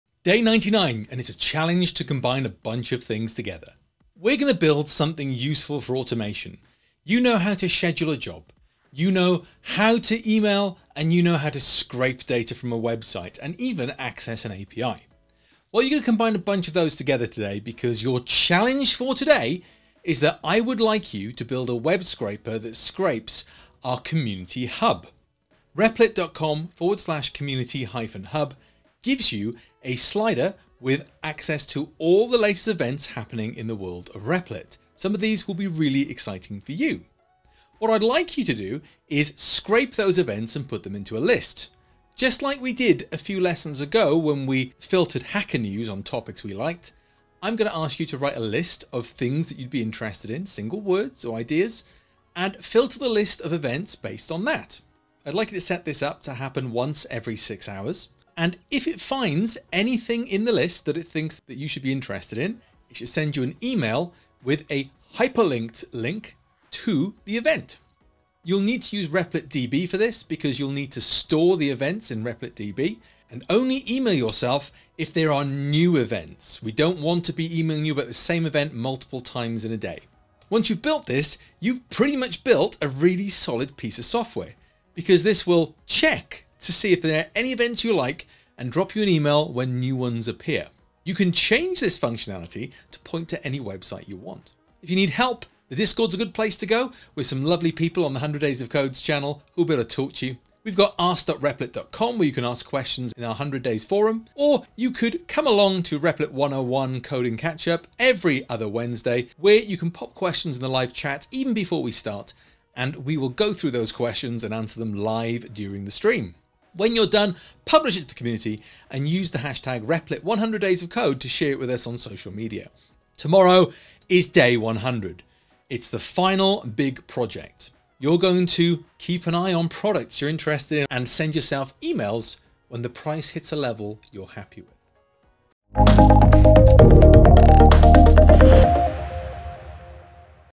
Day 99: 100 Days of Code: Project Day: Scrape the Hub
Today's challenge involves a combo scraper. Let's mix up emailing and scheduling and find the best Replit events for you to attend. Only ONE day to go!

00:00 Day 99 challenge
01:34 Get help
01:57 Share your code
02:03 Next up: The last day!